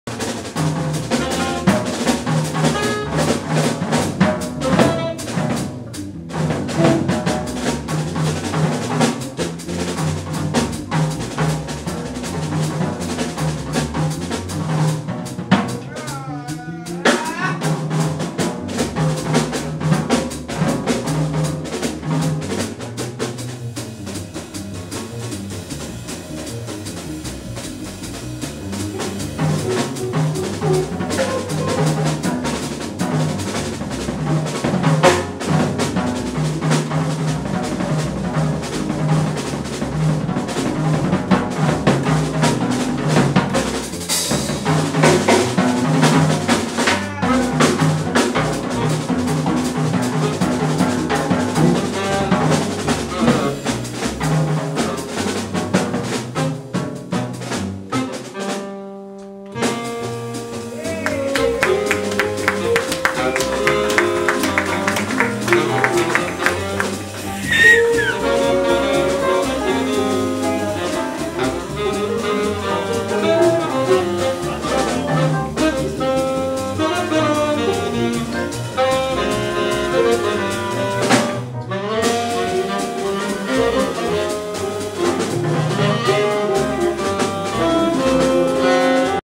This Quartet : François Rossi, batterie.
Extrait de la Jam session This Quartet du jeudi 12 mai 2011 au Planet Mundo Kfé (Marseille)